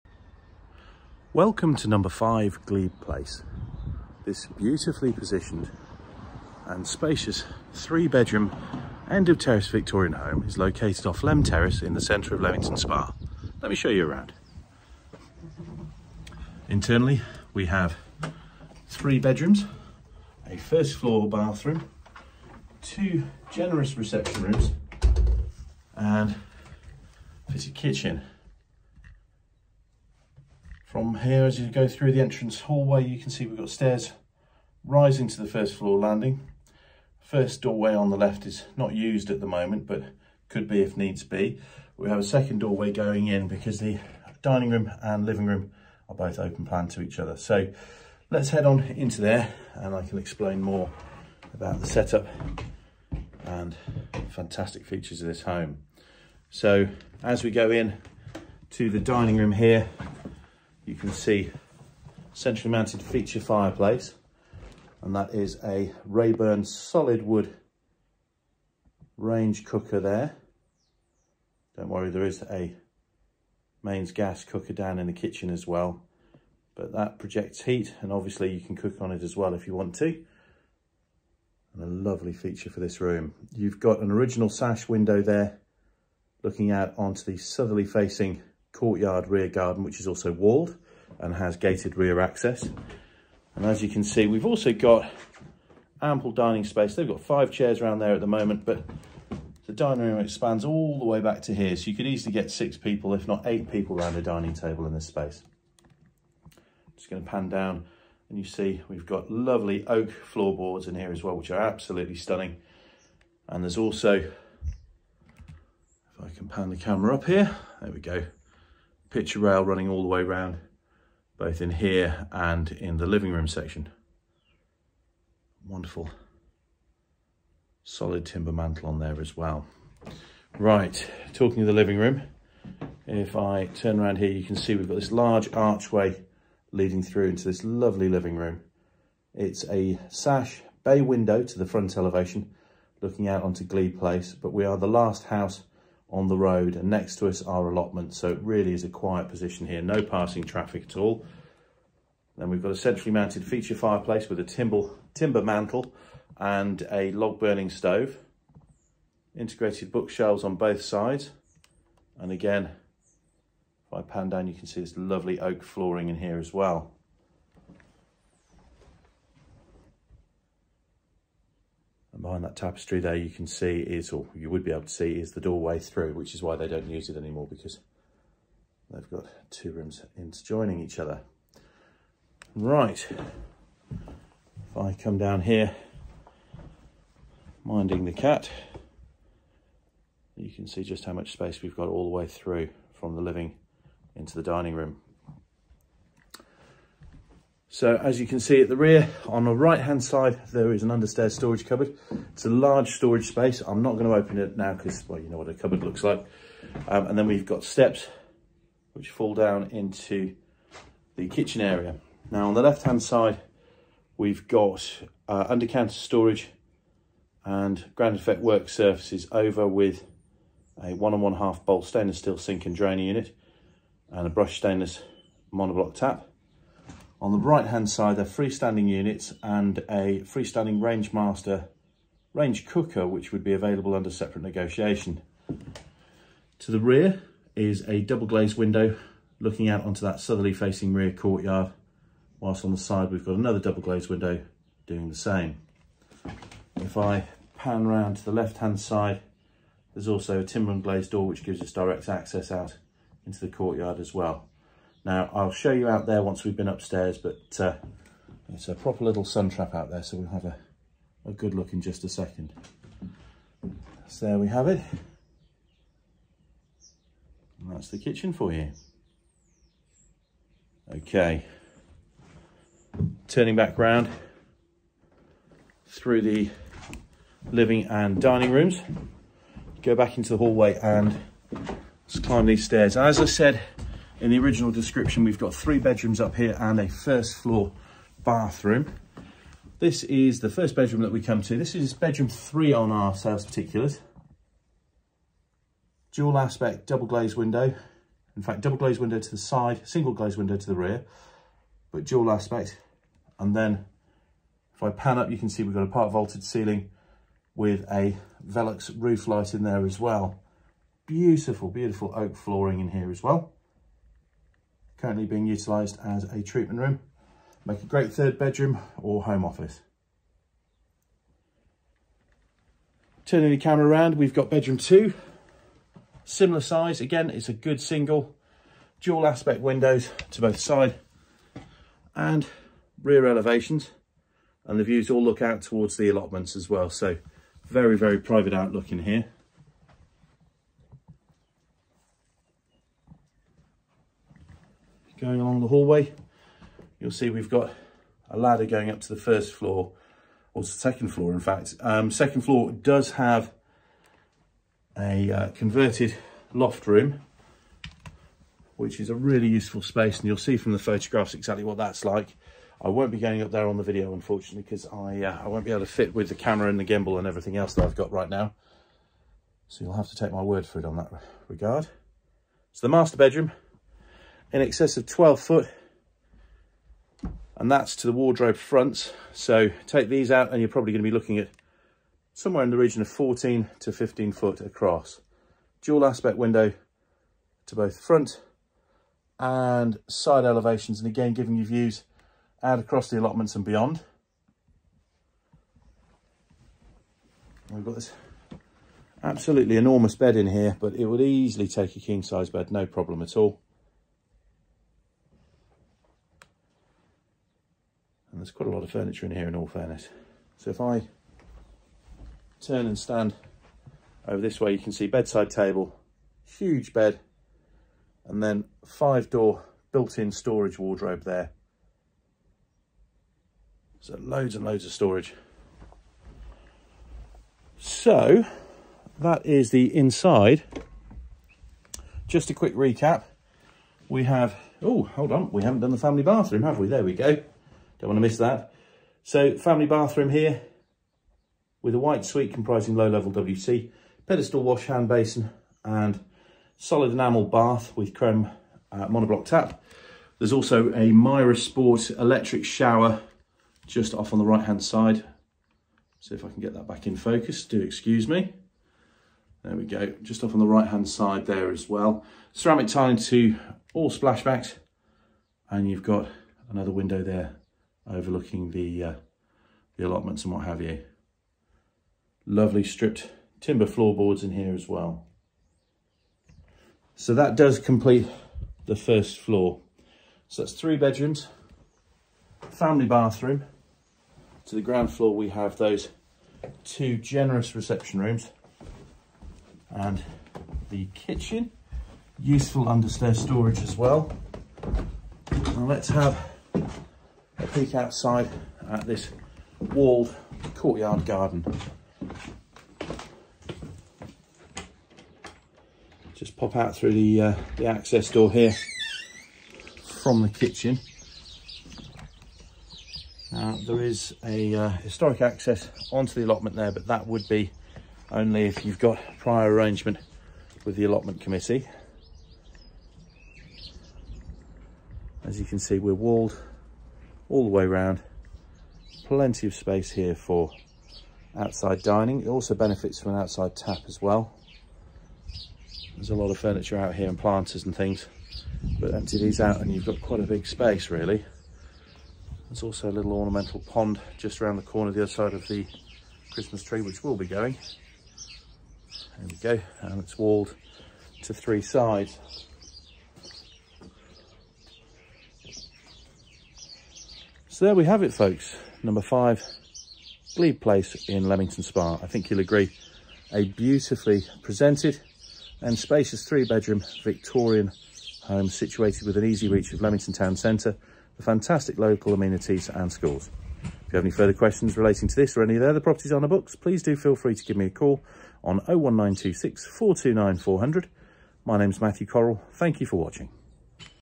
5 Glebe Place, Royal Leamington Spa - Three Bedroom, Traditional Terrace Home
A quintessentially Victorian end of terrace home located on this quite back water of Leam Terrace offering easy access to Leamington Town Centre and fantastic local amenities. With wonderful and traditional interior accommodation briefly comprising entrance hallway, living room, dining room, kitchen, three first floor bedrooms and bathroom. Outside is a south facing courtyard rear garden and internal viewing is highly recommended.